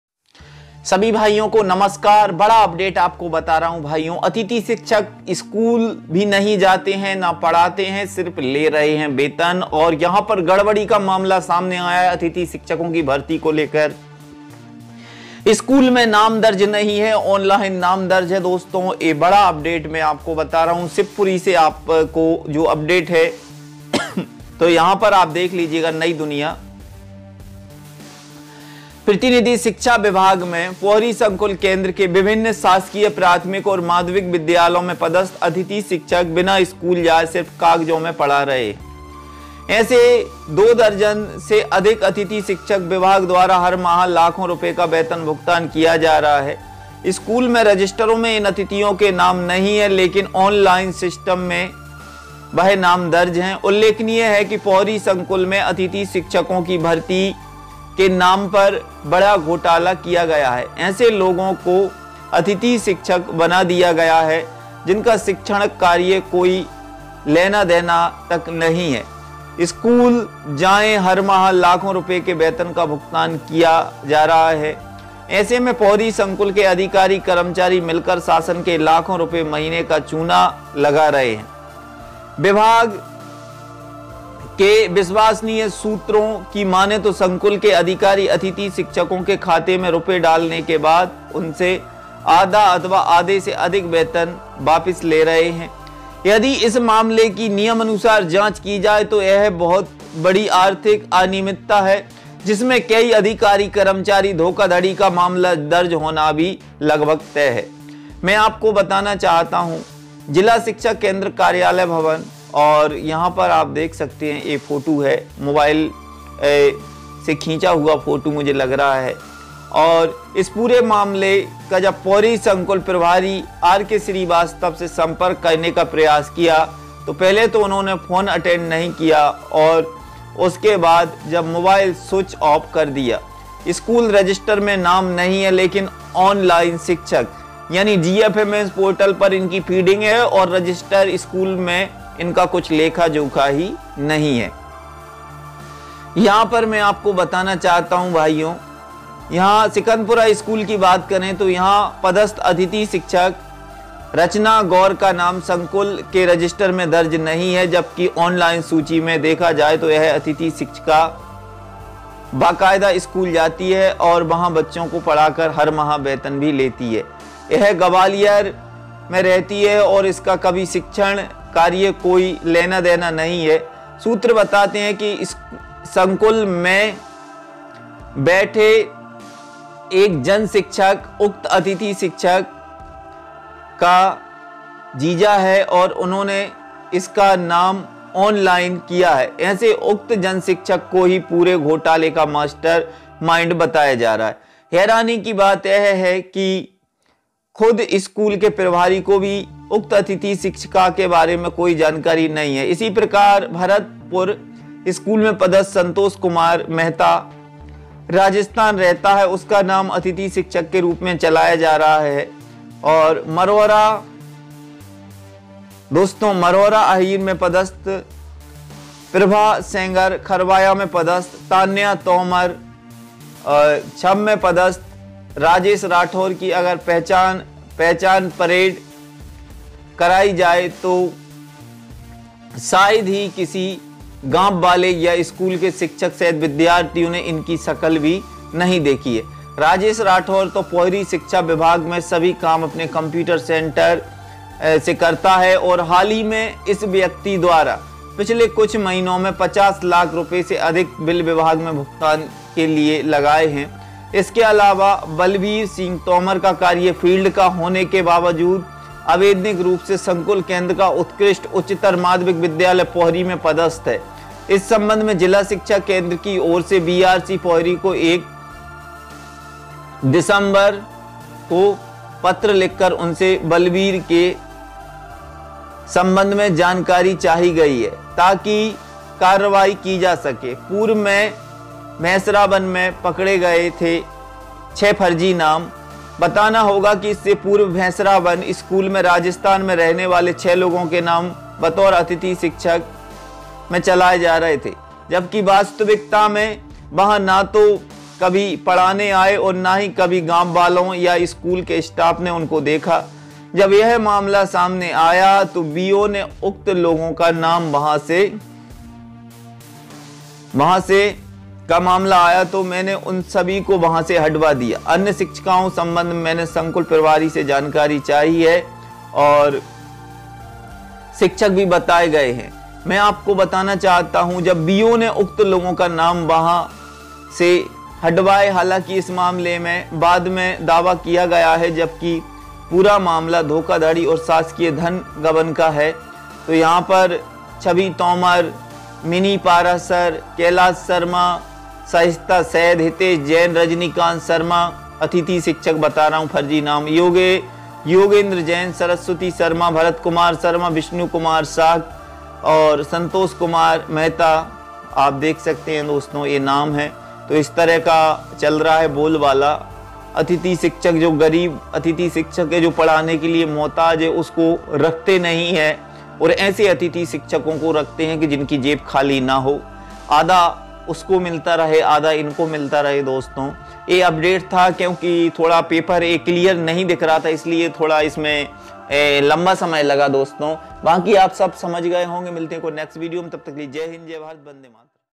अतिथि शिक्षक न स्कूल जाते हैं ना पढ़ाते हैं और ले रहे हैं वेतन
# फर्जी अतिथि शिक्षकों के नाम पकड़ाया